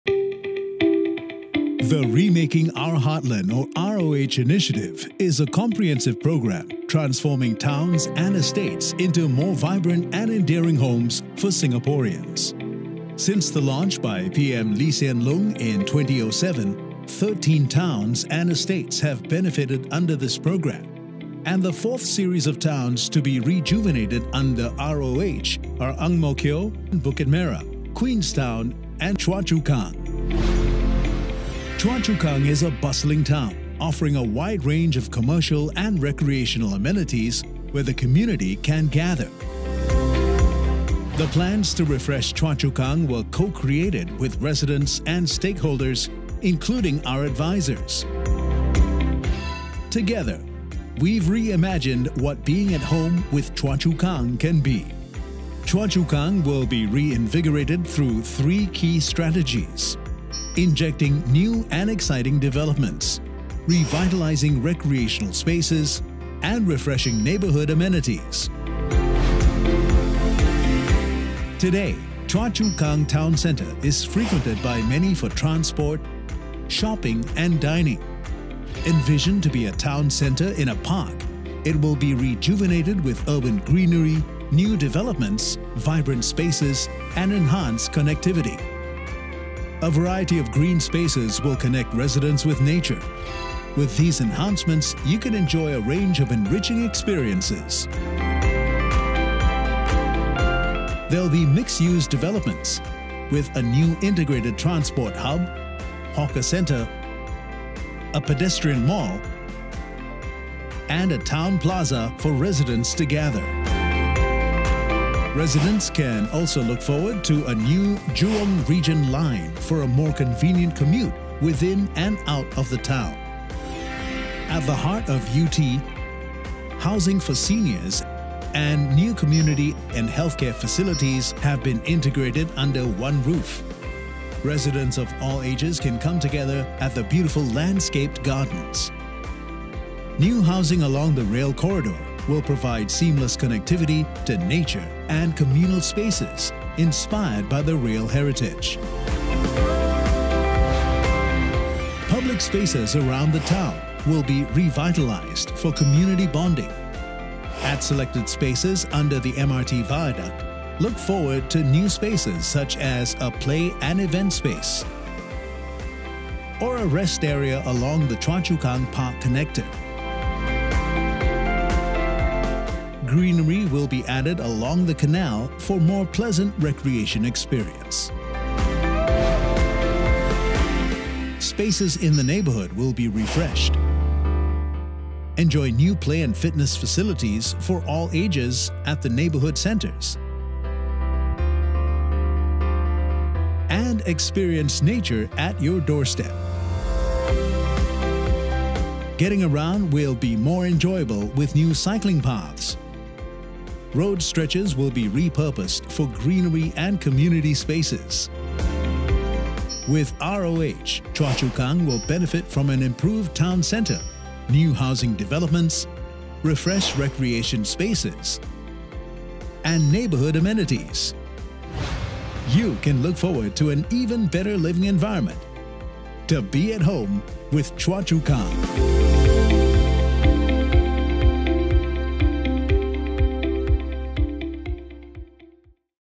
Remaking Our Heartland: Choa Chu Kang
Under ROH Programme, Choa Chu Kang will be rejuvenated with a refreshed town centre, upgraded neighbourhood centres and recreation spaces, enhanced green spaces and connectivity, as well as new housing developments with a stronger town identity.